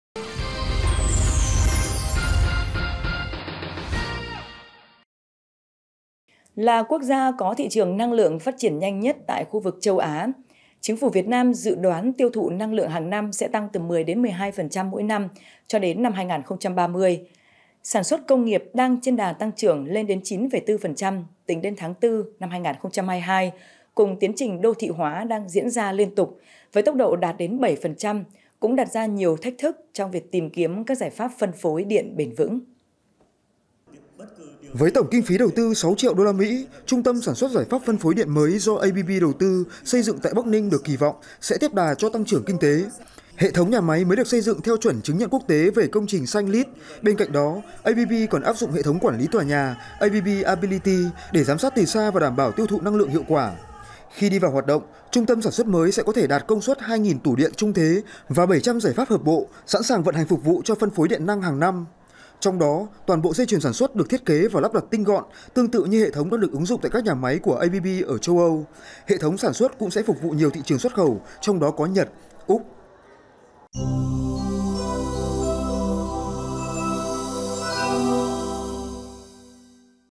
Giải pháp phân phối điện bền vững thúc đẩy phát triển kinh tế
Là quốc gia có thị trường năng lượng phát triển nhanh nhất tại khu vực Châu Á, Chính phủ Việt Nam dự đoán tiêu thụ năng lượng hàng năm sẽ tăng từ 10 – 12% mỗi năm cho đến 2030. Sản xuất công nghiệp đang trên đà tăng trưởng lên đến 9,4% tính đến tháng 4/2022 cùng tiến trình đô thị hóa đang diễn ra liên tục, với tốc độ đạt đến 7% cũng đặt ra nhiều thách thức trong việc tìm kiếm các giải pháp phân phối điện bền vững…